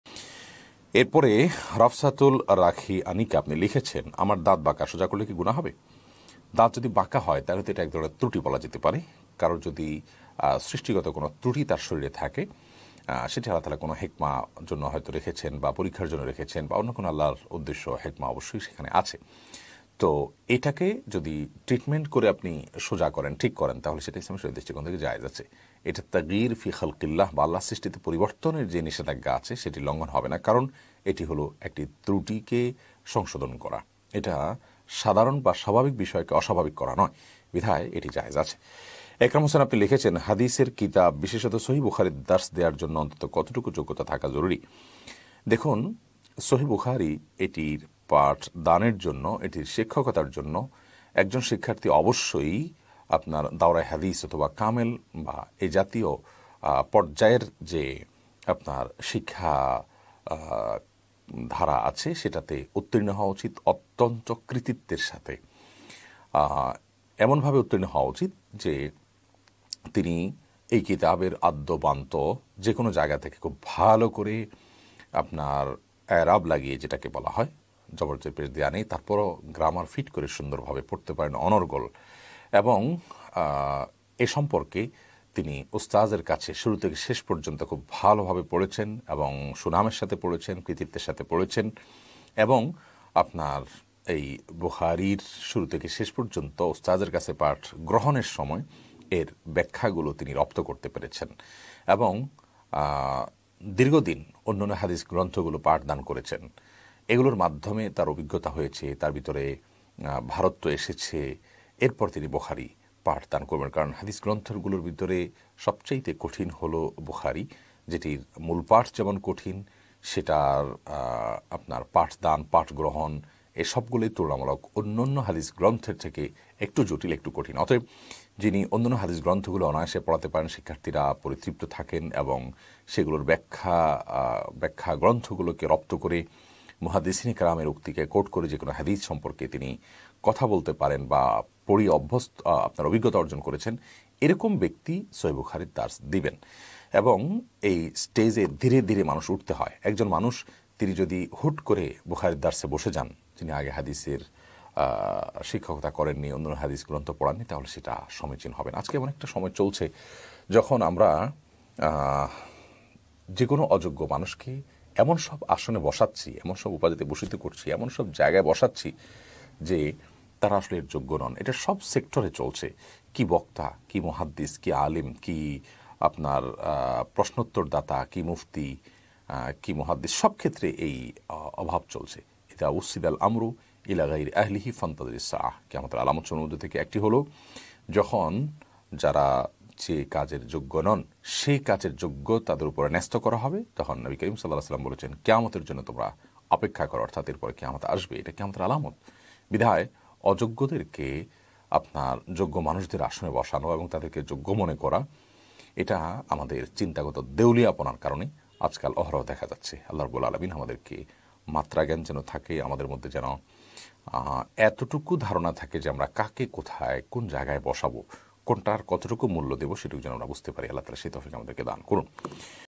আমার দাঁত বাঁকা সোজা করলে কি গুনাহ হবে!? শায়খ আহমাদুল্লাহ | New waz mahfil bd
Today we'll show

শাইখ আহমাদুল্লাহ
As-sunnah Foundation আস-সুন্নাহ ফাউন্ডেশন